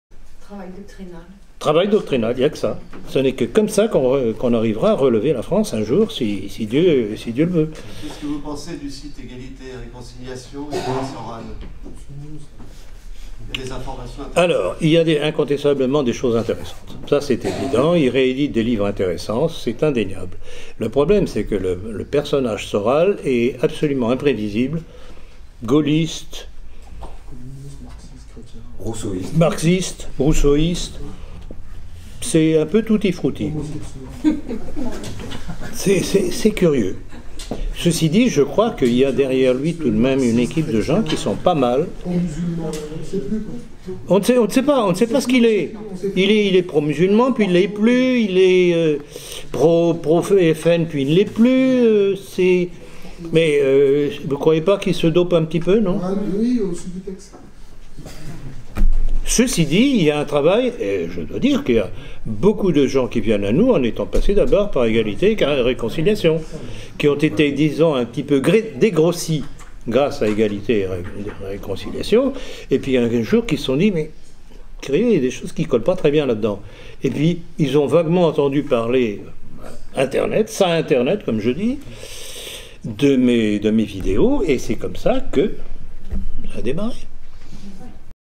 Philippe Ploncard d'Assac sur Alain Soral
Extrait de sa conférence "La politique de la terreur ou l'"Ordo ab chaos" en vue de la dictature mondiale messianique" (avril 2016)